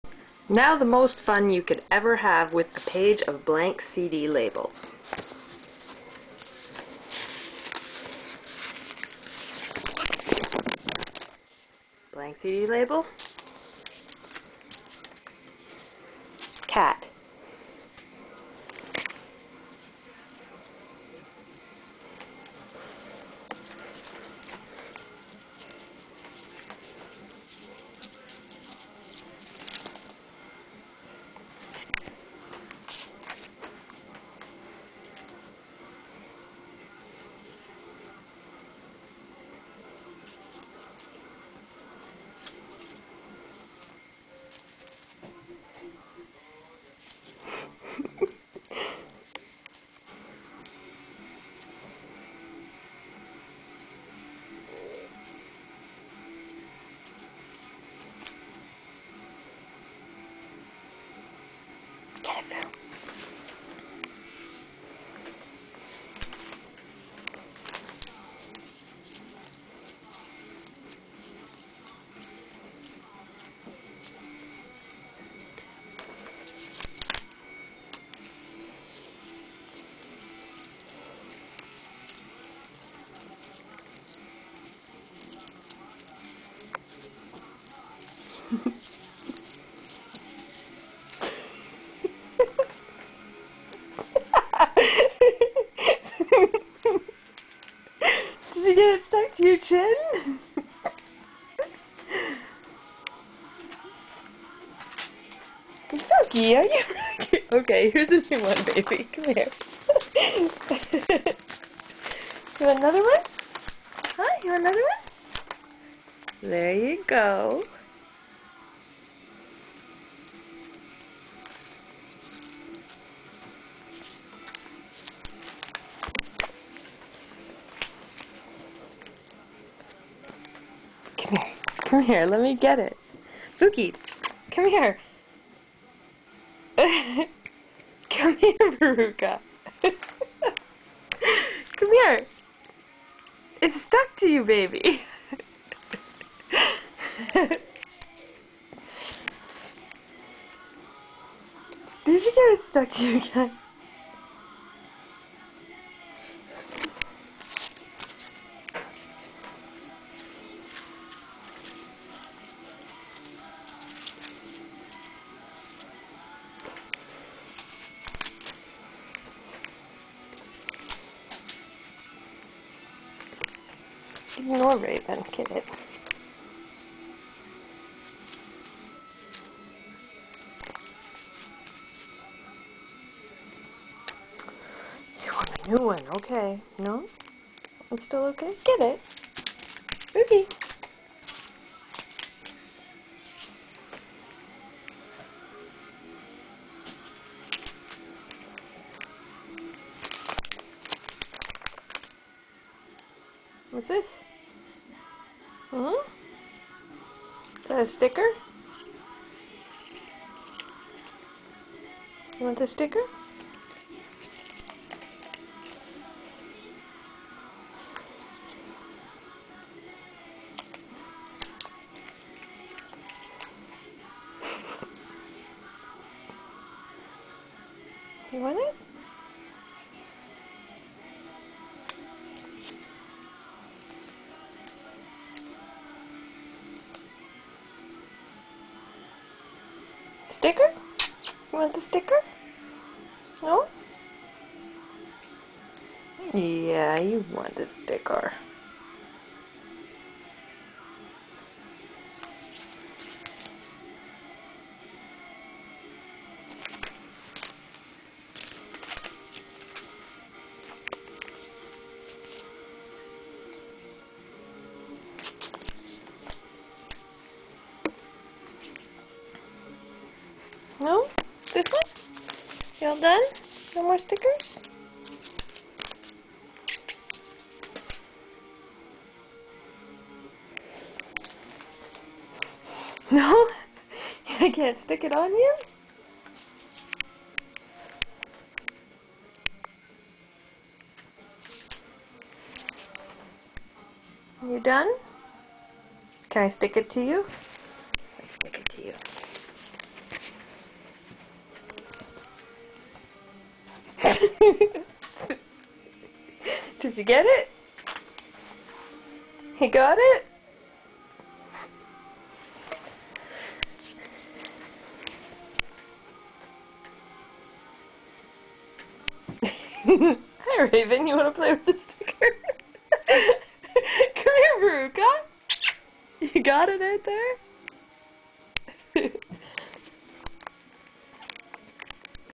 Put YOUR cat on a CD Label! ...literally
The most fun you could possibly have with a page of cd labels.

STICKY KITTY! (The best part is at 1minute 30 seconds)